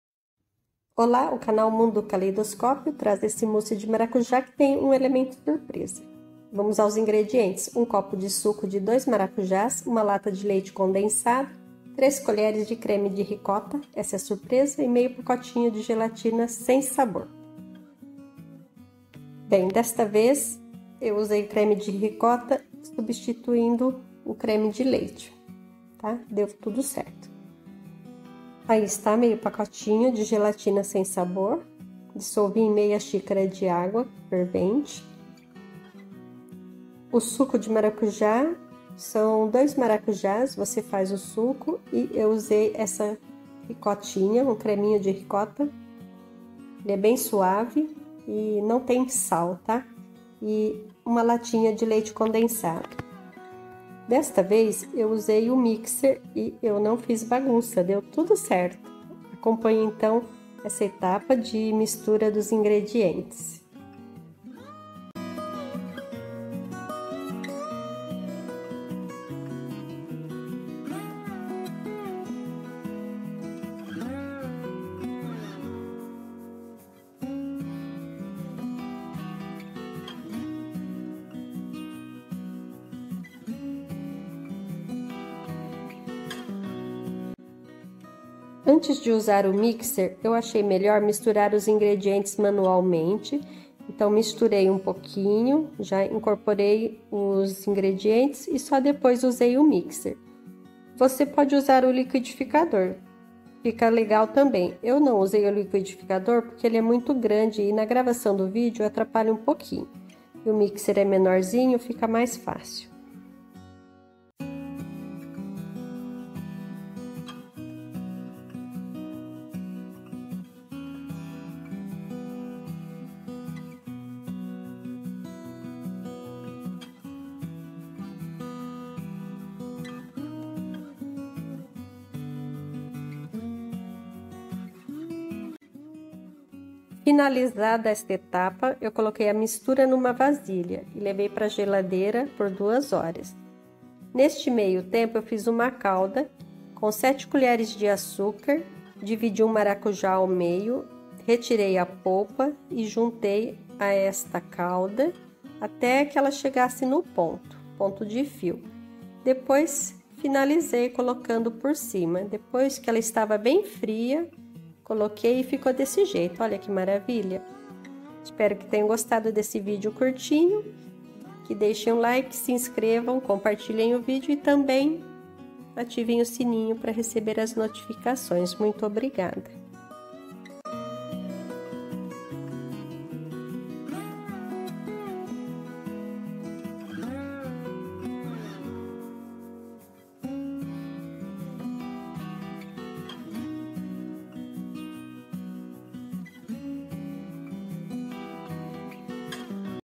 MOUSSE DE MARACUJÁ COM CREME DE RICOTA receitasdatina RECEITAS DE FAMÍLIA #1961
Mousse de Maracujá c/ Creme de Ricota
      Nesse vídeo trazemos até vocês uma receita de mousse de maracujá. Uma sobremesa deliciosa e refrescante.

INGREDIENTES
                               - Suco de 02 maracujás
                               - 01 lata de leite condensado
                               - 1/2 pacote de gelatina sem sabor (dissolvida em água fervente)
                               - 03 colheres de creme de ricota.

                  MODO DE PREPARO:
                                - Junte os ingredientes e  misture bem,  bata no mixer ou liquidificador e leve para                gelar.
CALDA
                                - 07 colheres de açúcar,
                               -  Polpa de 1/2 maracujá.

                    MODO DE PREPAO:
                            - Leve o açúcar ao fogo baixo até derreter,  adicione então meio copo de água e a polpa do maracujá. Deixe reduzir até a calda ficar em ponto de fio.